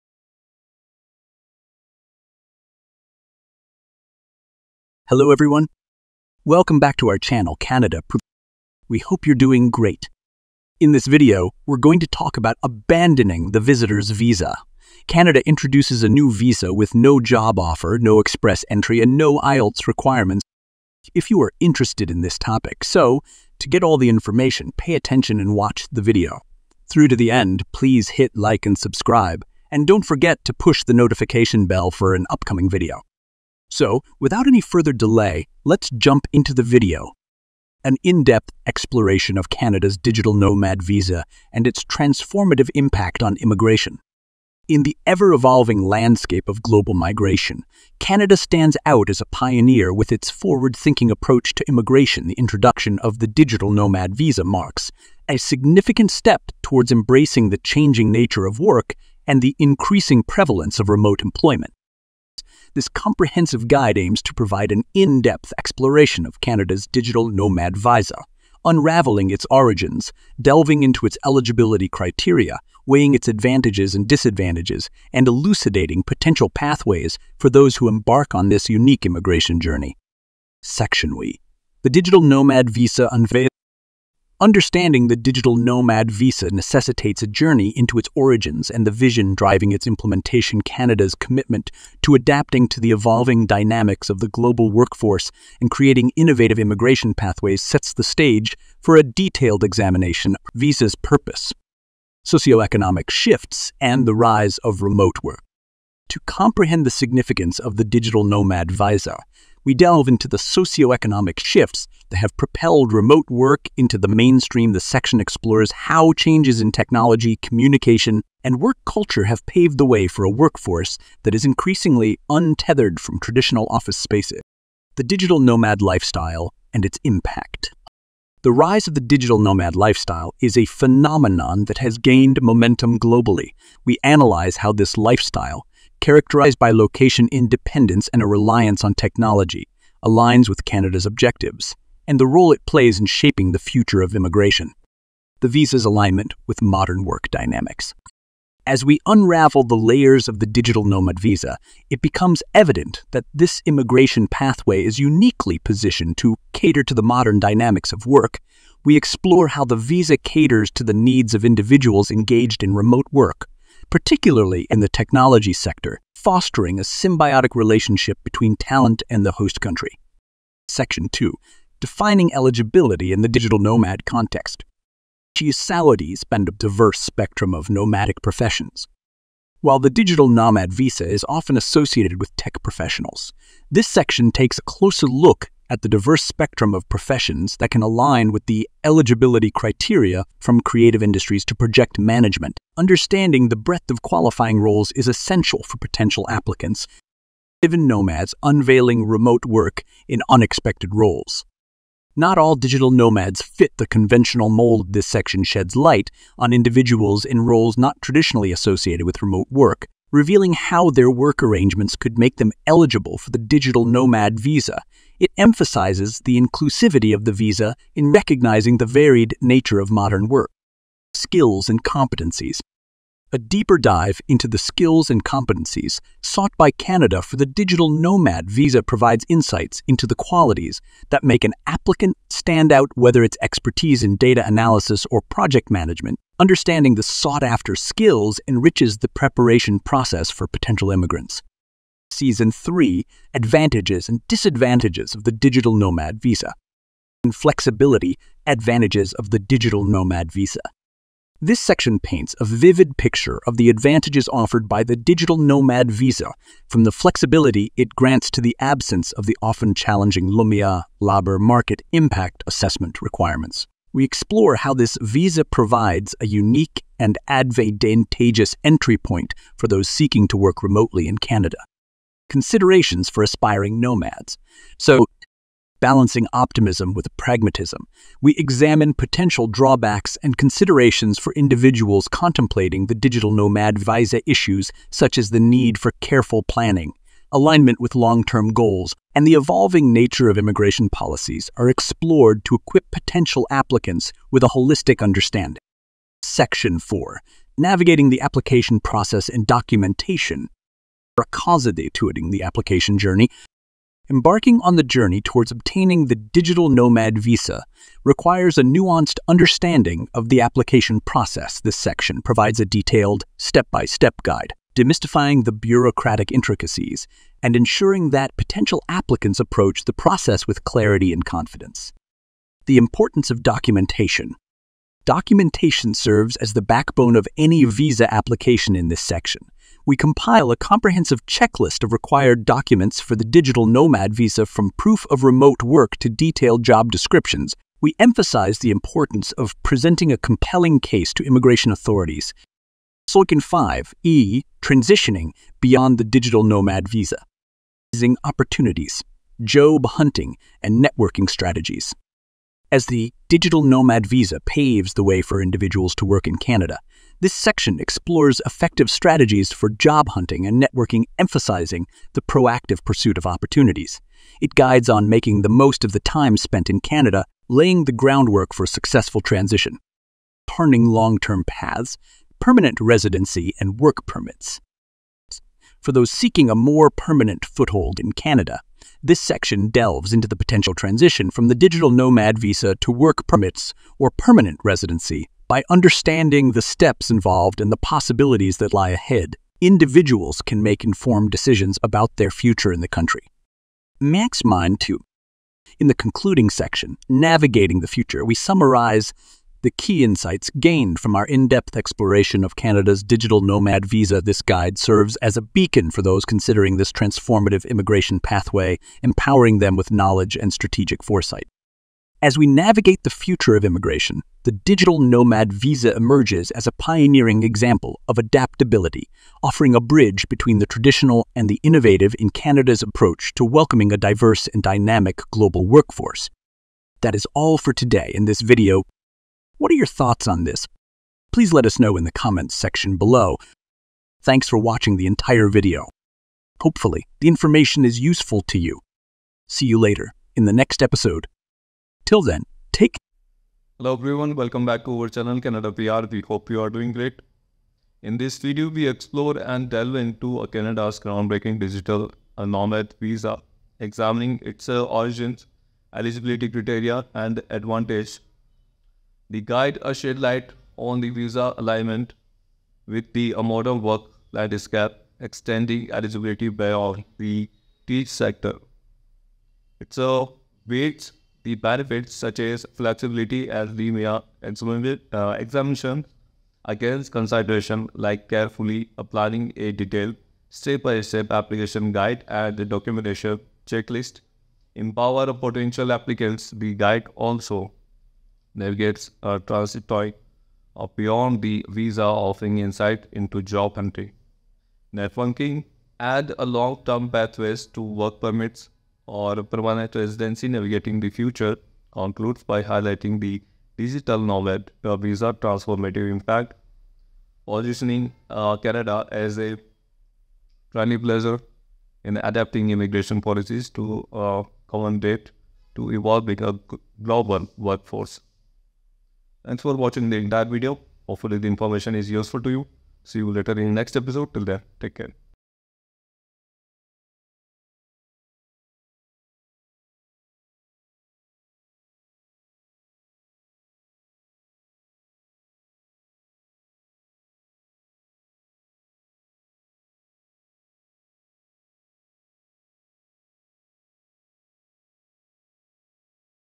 Canada Announces NEW Visa : No Job Offer, No Express Entry & No IELTS Required!
Sign in to your account.

Click the link to apply for permanent residence under Express Entry.
When filling out the form:

Make sure you submit all of the required documents.
All of your answers must be complete and true.
Make sure all the mandatory fields are completed, or you won’t be able to submit your application.
You can save your information on the form and go back as often as you need to.